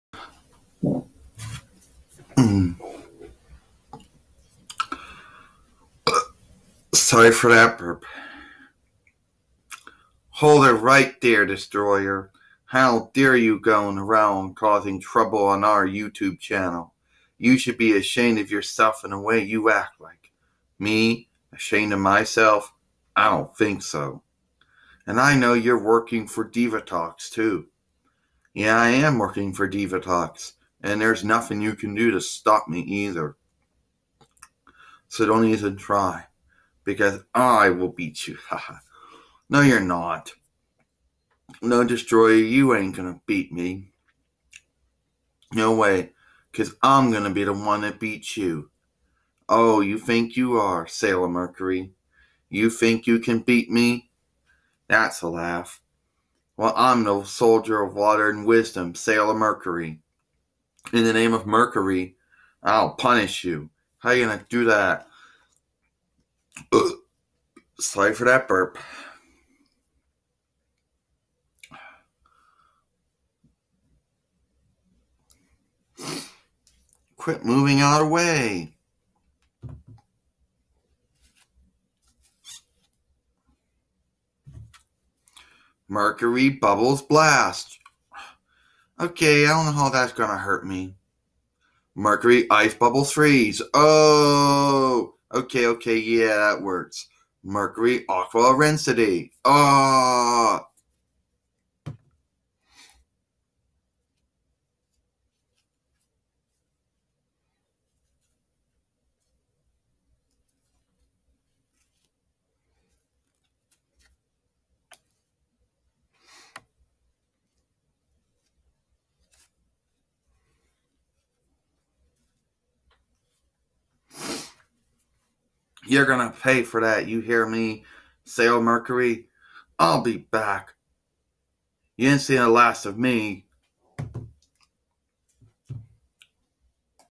SAILOR MERCURY VS DESTROYER
SAILOR MERCURY FIGHT DESTROYER WHO IS WORKING FOR DIVATOX AND IS TRYING TO KILL SAILOR MERCURY ON YOU TUBE